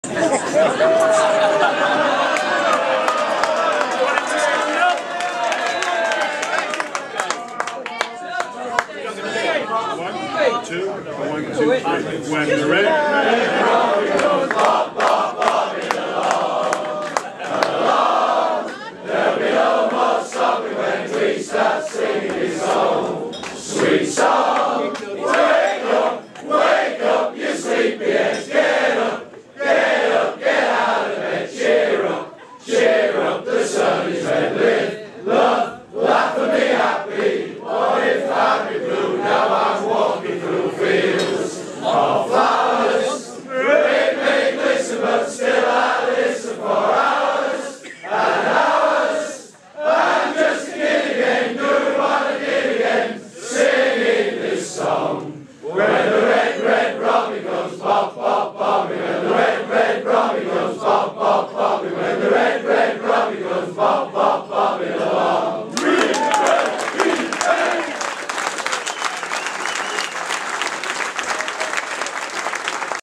Hull KR Fans Forum Sing-a-long
Paul Cooke, Justin Morgan, Gwilym Lloyd, Neil Hudgell and Paul Lakin all singing along at the fans forum